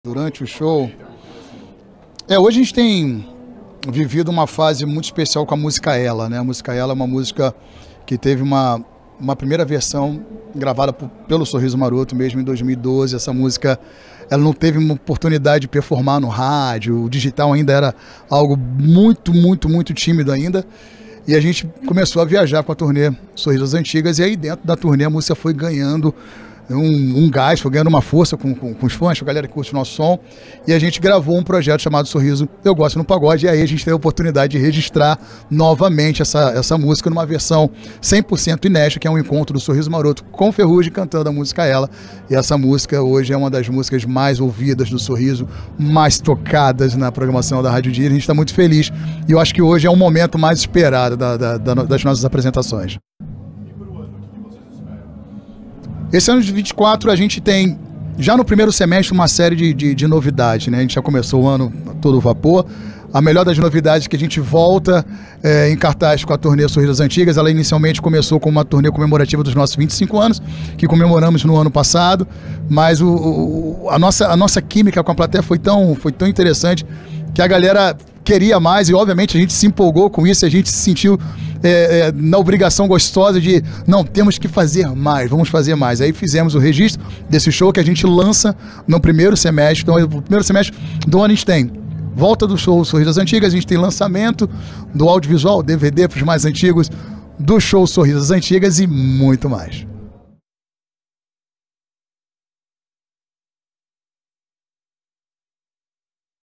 Entrevista Sorriso Maroto - Rádio Disney
A Rádio Disney fez essa entrevistas exclusiva com o Sorriso Maroto!
Trocamos uma ideia muito legal no camarim no evento Belo Sorriso de 2024! Confere aí!

Artistas: Bruno Cardoso; Sérgio Jr, Cris Oliveira, Vinícius Augusto, Fred Araújo.
Produção: Lucas Kawasaki e Rodrigo Mello
Gravação: Lucas Kawasaki
Edição: Lucas Kawasaki
Áudio: Rodrigo Mello
Direção: Danilo Rocha e Rodrigo Girassol